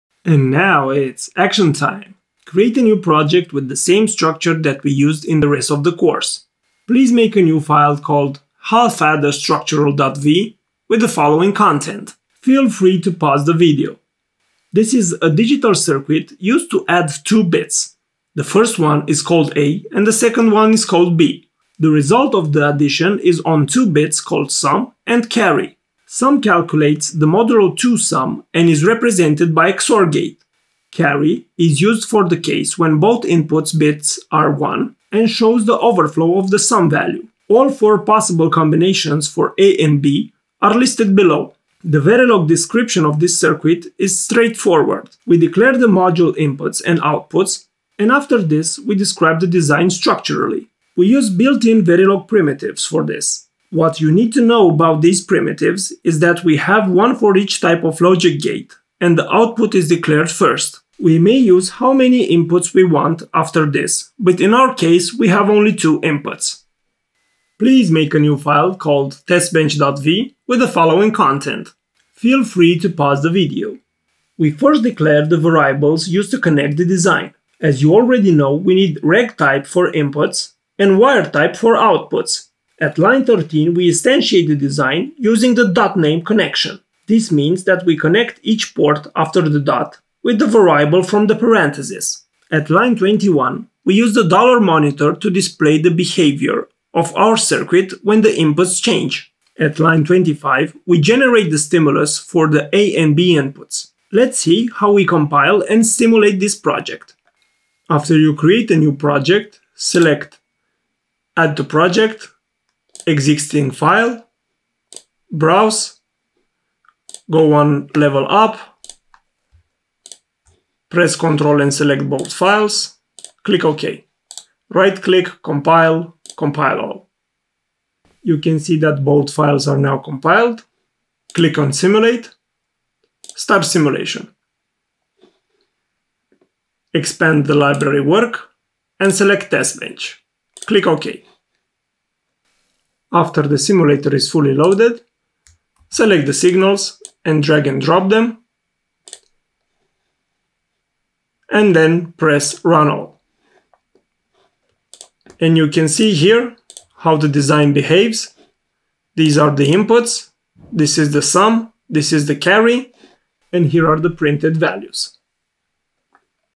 Design a Verilog half adder - Verilog project for beginners
Implement a simple Verilog half adder circuit that calculates the sum of two bits.

You also get to implement the testbench for it and simulated it using Modelsim Free Edition.

This video is part of my easy, practical and, complete Udemy course
Find out how easy Verilog is!